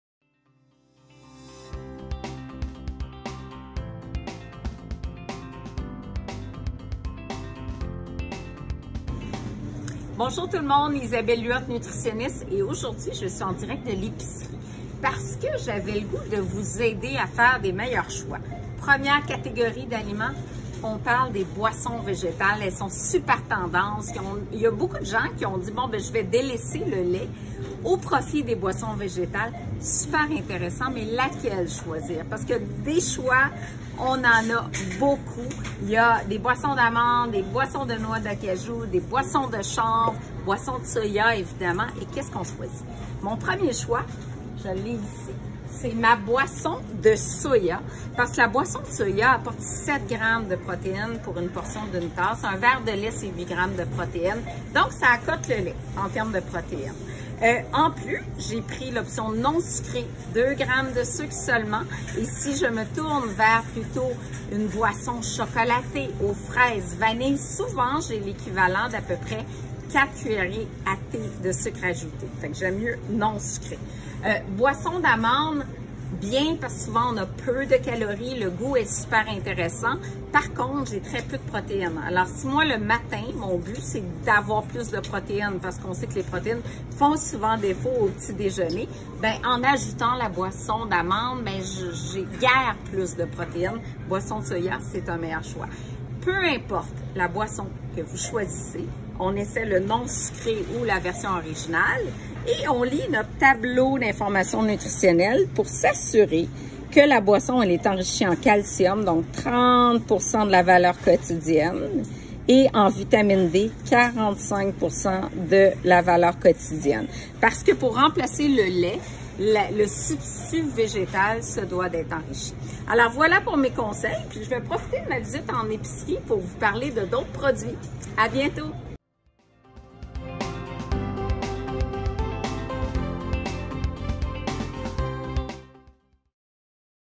Choisir la bonne boisson végétale
On délaisse le lait de vache? La nutritionniste Isabelle Huot fait le tour avec nous des options au rayon des boissons végétales.